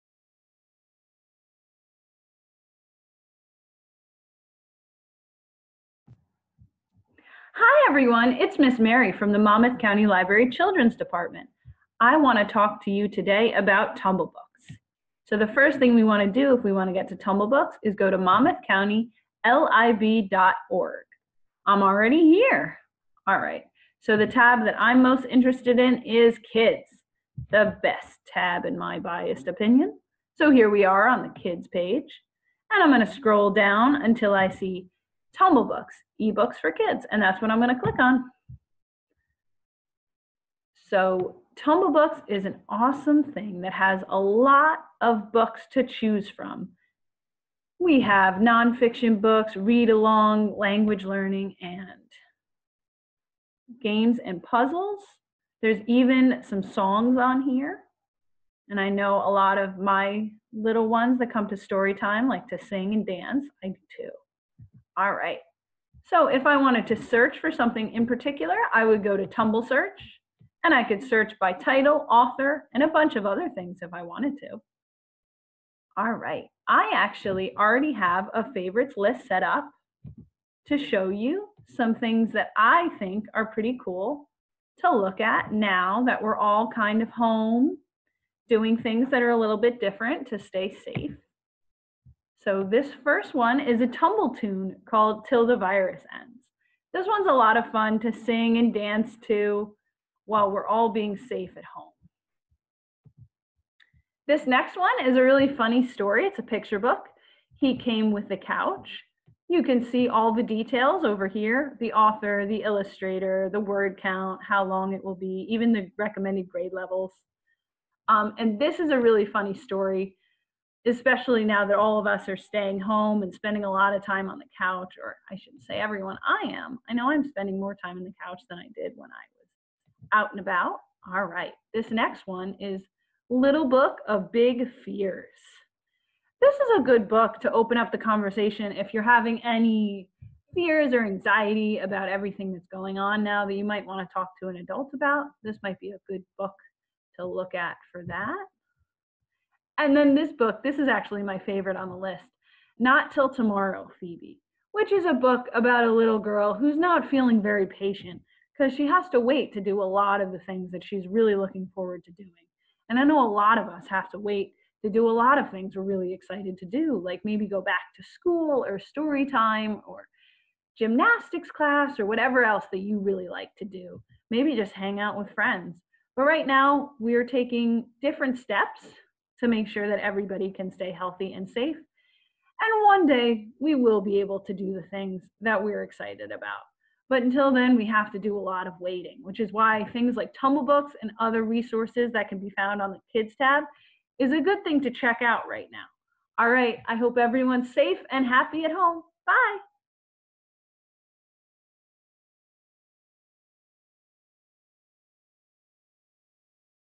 How To Find & Use Tumblebooks with Librarian Mary, Monmouth County Library
Tumblebooks is a tremendous resource offered for free by the Monmouth County Library. In this video, you can follow the ez path offered by Mary and play, read or be read to at this fascinating site.
for more reading and fun for all ages.